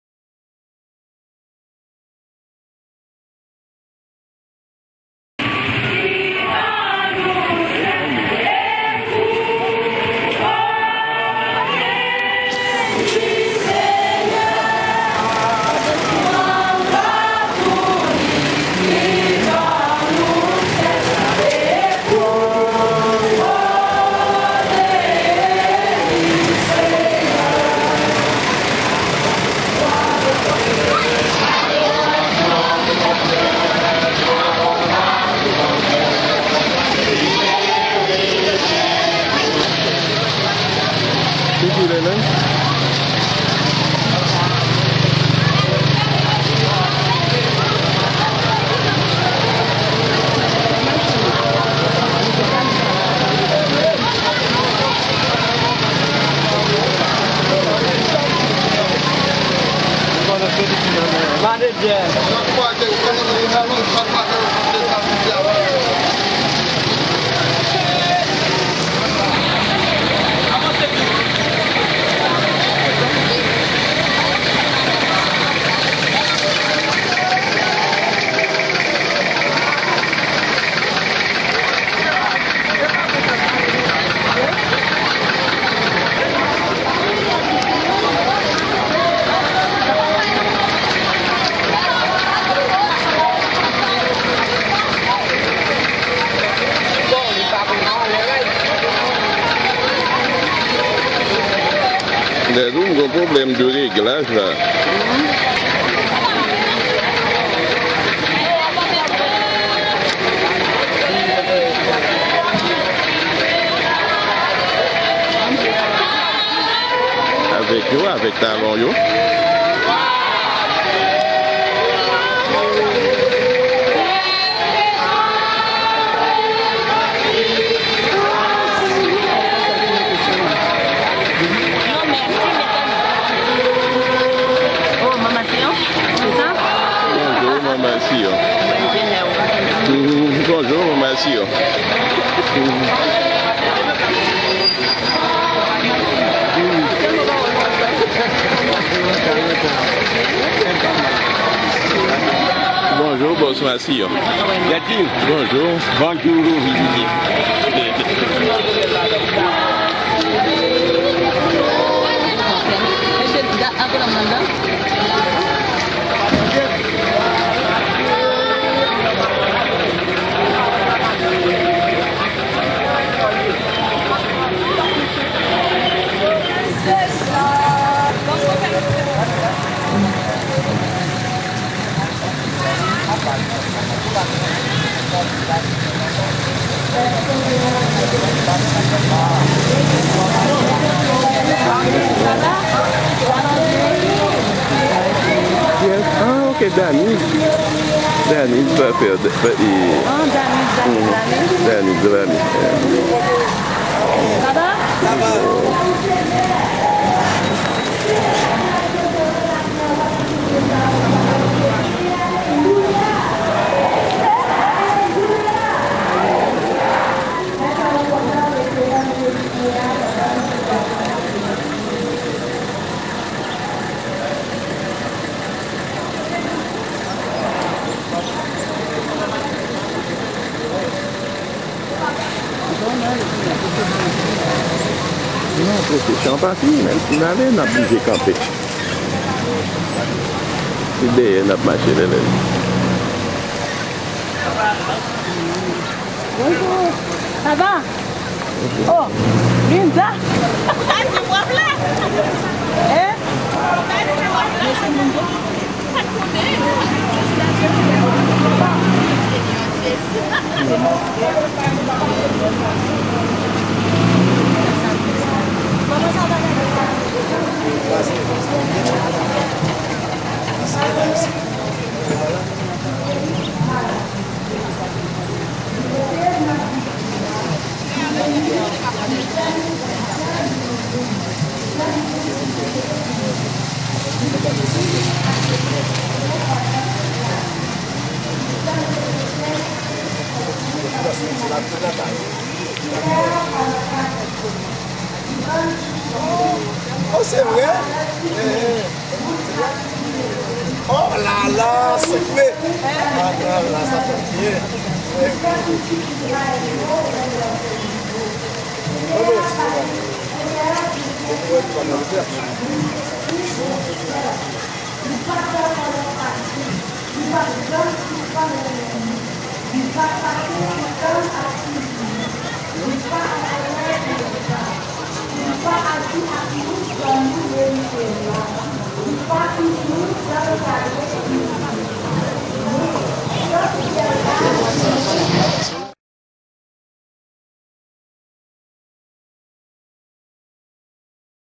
160e Église Baptiste Salem de Dondon (Haïti) - défilé 6 mai 2012 - Grand'rue.wmv
Défilé du dimanche 6 mai 2012 à Dondon (Haïti) - commémoration des 160 ans d'existence de l'Église Baptiste de Dondon, fondée le 5 mai 1852.